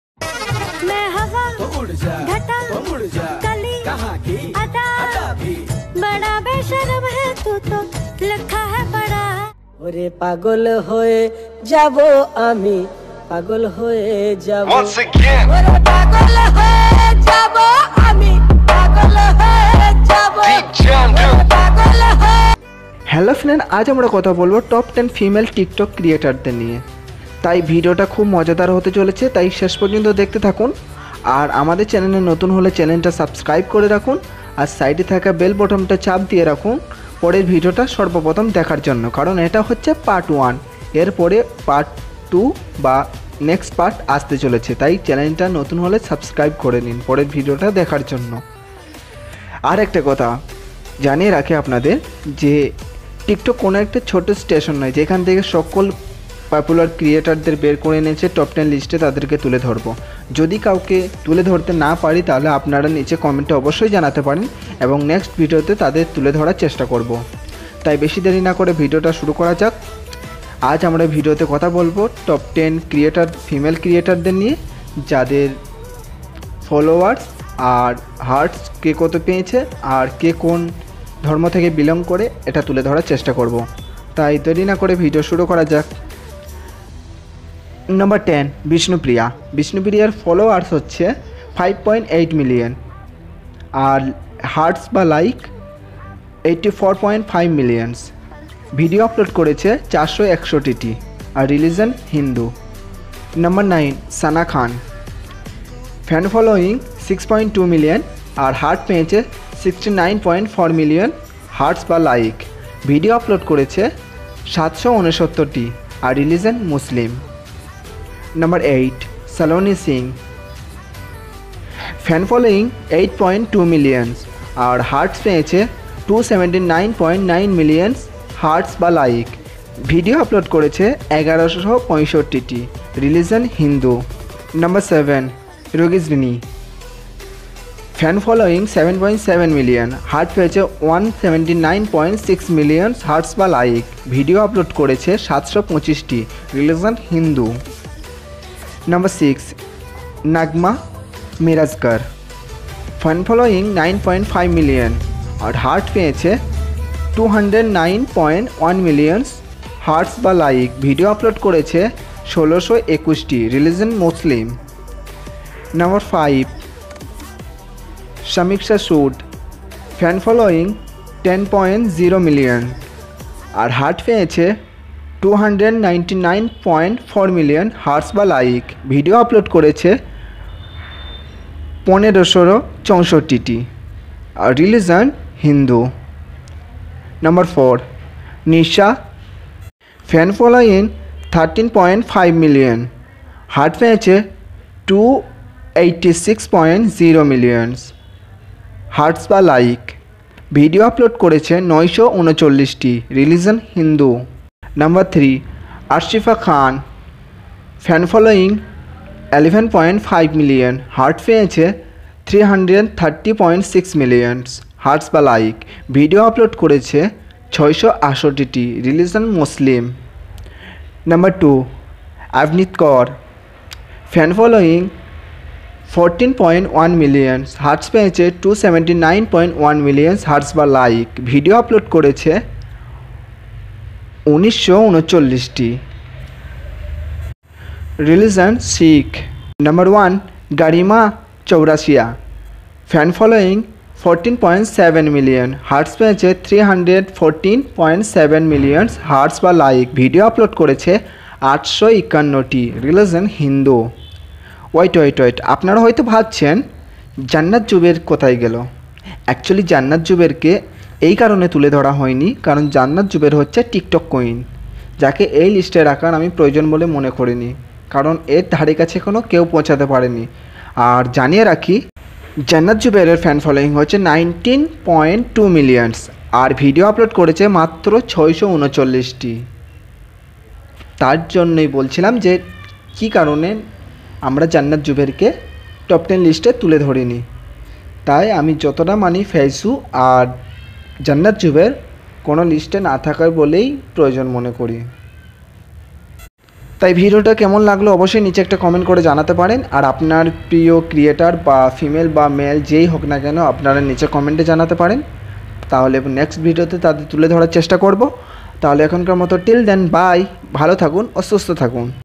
কে কোন ধর্মের | top tik tok star | jannat_zubair | saloni_sing | gima_arshi, Avneet_Kaur_13 | Riyaz
hello friends welcome to filmi talk bangla YouTube channel
ami safik ajj apander samne kichu top tik tok Creator's der tule chesta korechi
jodi list kono vul thake comments a janate paren
about.
top ten female tik tok super star der niye ekta choto video
ai video te jante parben kon Creator's er koto fan following ar koto heart's peyeche
ar kon star kon dhormo theke belong kore
tai amader list anujayi
10. vishnupriya__14 (shizuka)
9. sana_Khan
8. saloni_sing
7. Rugeesvini
6. nagmaa_mirajkar
5.  sameeksha_sud
4. Nisha
3. arishfa_khan
2. Avneet_kour
1. garima_chaurashi (gima_arshi)